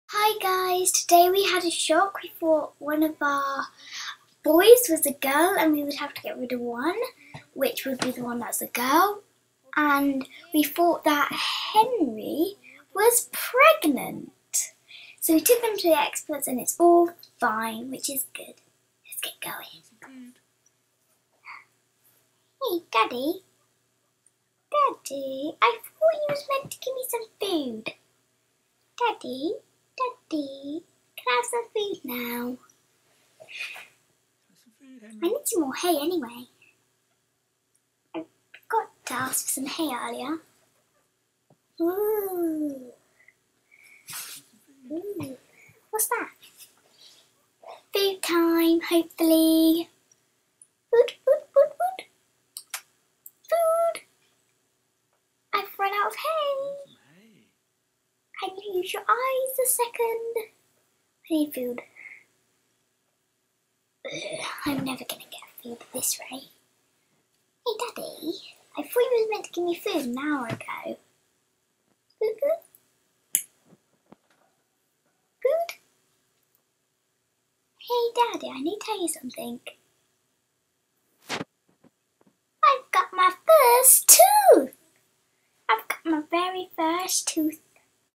Shock/Horror Guinea Pig Boys maybe Girls!
This week was a shocker, we thought Henry and Lewis were actually Henryetta and Louise! Took them to an expert and all is ok, false alarm.

Our spoof GUINEA PIG STUDIOS 'short films' are